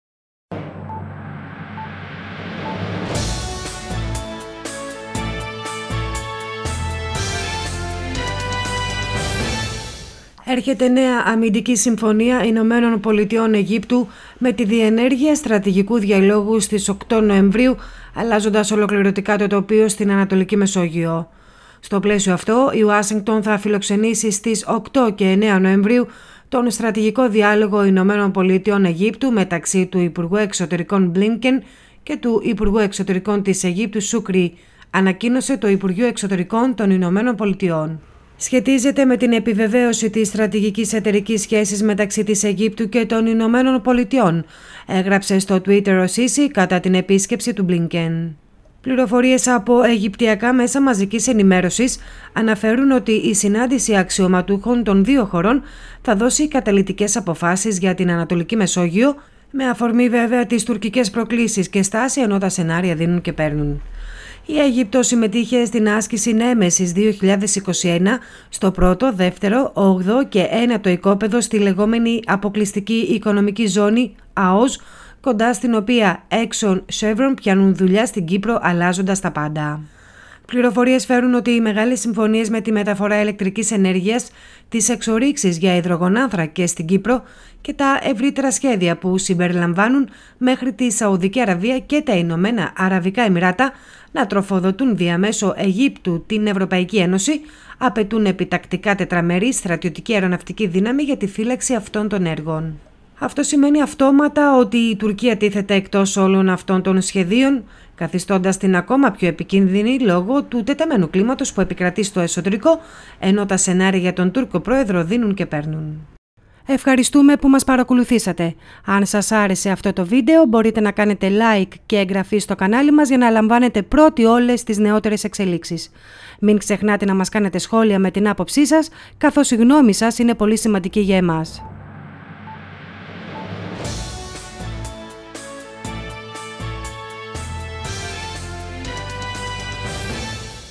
Έρχεται αμυντική συμφωνία Αιγύπτου-ΗΠΑ μετά την "Nemesis"!
Όλα αλλάζουν στην Α.Μεσόγειο και εισερχόμαστε σε νέα εποχή.
Έρχεται νέα αμυντική συμφωνία ΗΠΑ-Αιγύπτου, με την διενέργεια στρατηγικού διαλόγου στις 8 Νοεμβρίου, αλλάζοντας ολοκληρωτικά το τοπίο στην Α.Μεσόγειο.
Στο πλαίσιο αυτό η Ουάσιγκτον θα φιλοξενήσει στις 8 και 9 Νοεμβρίου τον στρατηγικό διάλογο ΗΠΑ-Αιγύπτου μεταξύ του υπουργού Εξωτερικών Antony Blinken και του υπουργού Εξωτερικών της Αιγύπτου Sameh Shoukry, ανακοίνωσε το Υπουργείο Εξωτερικών των ΗΠΑ.
“Σχετίζεται με την επιβεβαίωση της στρατηγικής εταιρικής σχέσης μεταξύ της Αιγύπτου και των ΗΠΑ», έγραψε στο Twitter ο Σίσι κατά την επίσκεψη του Blinken.

Πληροφορίες από αιγυπτιακά ΜΜΕ αναφέρουν ότι η συνάντηση αξιωματούχων των δύο χωρών, θα δώσει καταλυτικές αποφάσεις για την Α.Μεσόγειο, ( με αφορμή βεβαία τις τουρκικές προκλήσεις και στάση ενώ τα σενάρια δίνουν και παίρνουν).
Η Αίγυπτος συμμετείχε την άσκηση «ΝΕΜΕΣΙΣ 2021» στο 1ο, 2ο, 8ο και 9ο οικόπεδο στη λεγόμενη «Αποκλειστική Οικονομική Ζώνη» (ΑΟΖ), κοντά στην οποία EXXON-CHEVRON πιάνουν δουλειά στην Κύπρο αλλάζοντας τα πάντα.
Πληροφορίες φέρουν ότι οι μεγάλες συμφωνίες με τη μεταφορά ηλεκτρικής ενέργειας, τις εξορύξεις για υδρογονάνθρακες στην Κύπρο, και τα ευρύτερα σχέδια που συμπεριλαμβάνουν μέχρι τη Σ.Αραβία και τα ΗΑΕ να τροφοδοτούν δια μέσω Αιγύπτου την ΕΕ, απαιτούν επιτακτικά Τετραμερή στρατιωτική αεροναυτική δύναμη για την φύλαξη αυτών των έργων.
Αυτό σημαίνει αυτόματα ότι η Τουρκία, τίθεται εκτός όλων αυτών των σχεδίων, καθιστώντας την ακόμα πιο επικίνδυνη, λόγω του τεταμένου κλίματος που επικρατεί στο εσωτερικό, ενώ τα σενάρια για τον Τούρκο πρόεδρο δίνουν και παίρνουν.